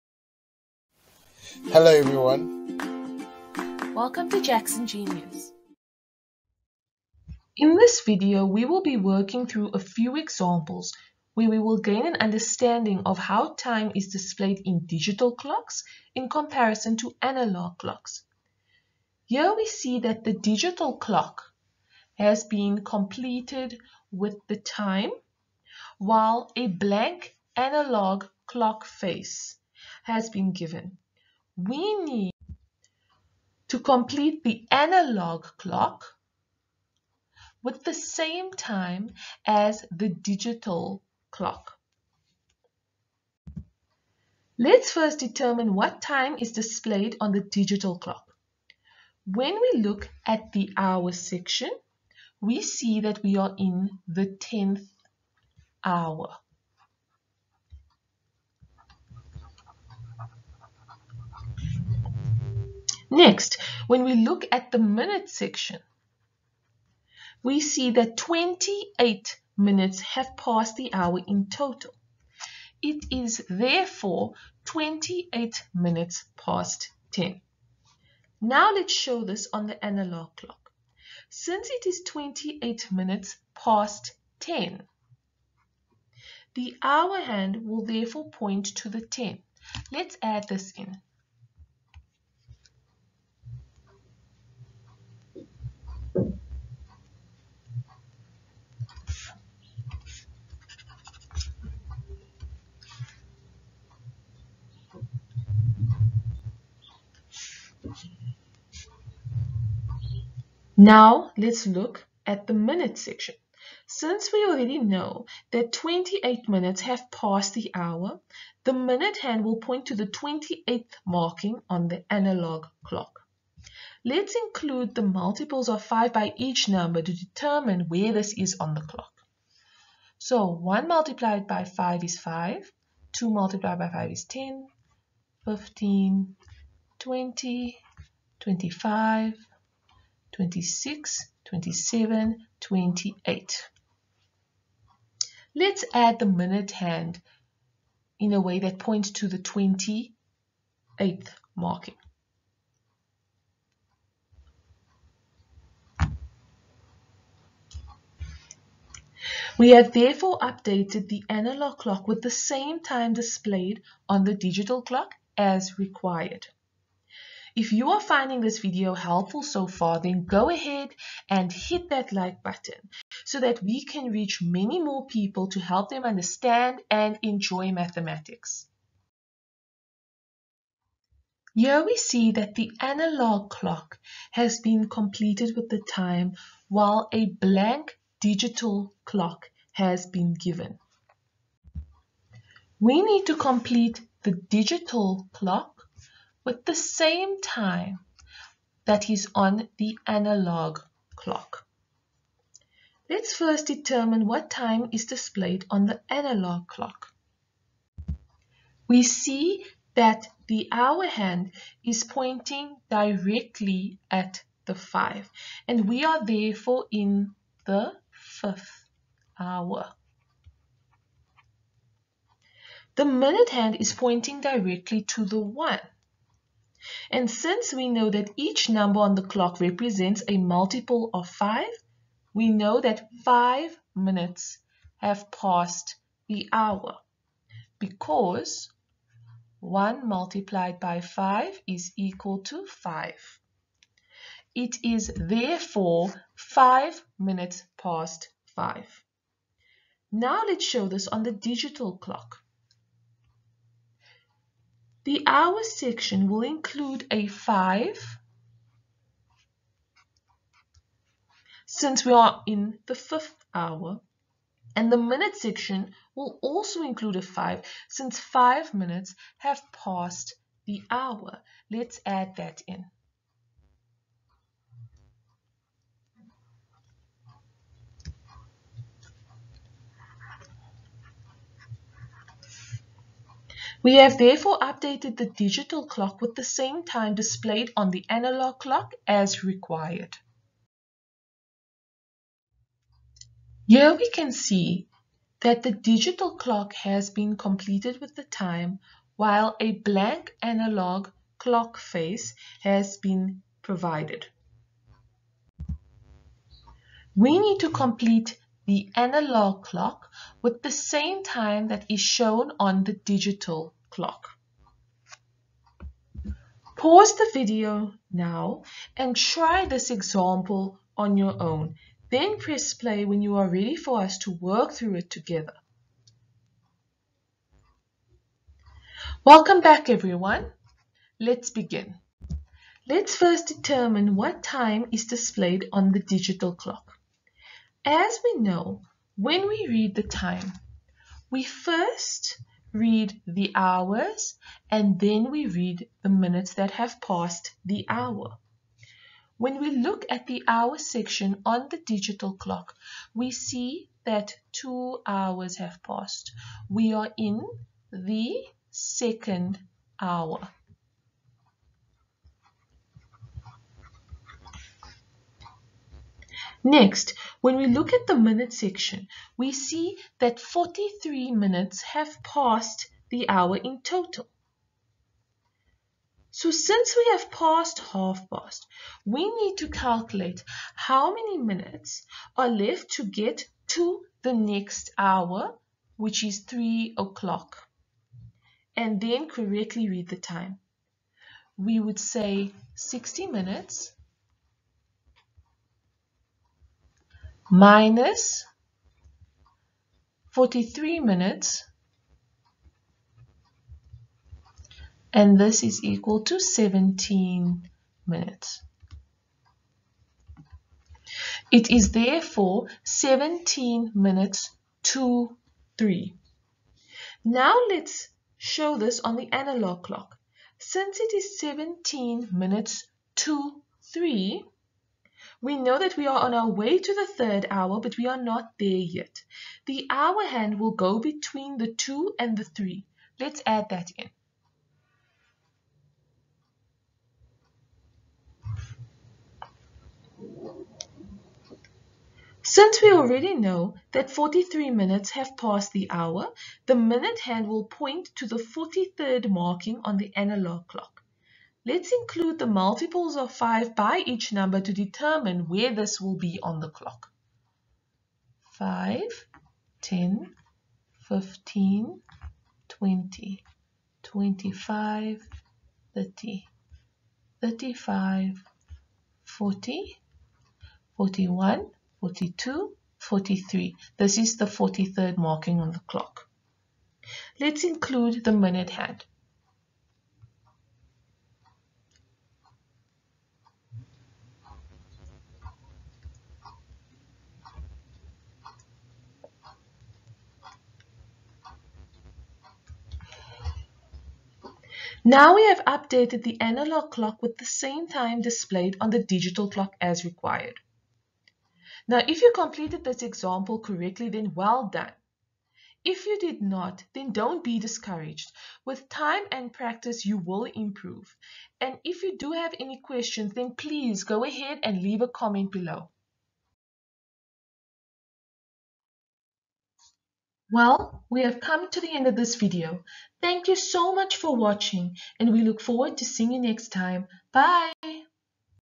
Digital Clock vs Analogue Clock Examples – Telling Time I Jackson Genius
In this video we will be working through a few examples where we will gain an understanding of how time is displayed in digital clocks in comparison to analogue clocks. Let’s work through these examples together.

Chapters:
00:00 Introduction
00:06 Example 1
03:00 Example 2
04:15 Example 3
08:38 Conclusion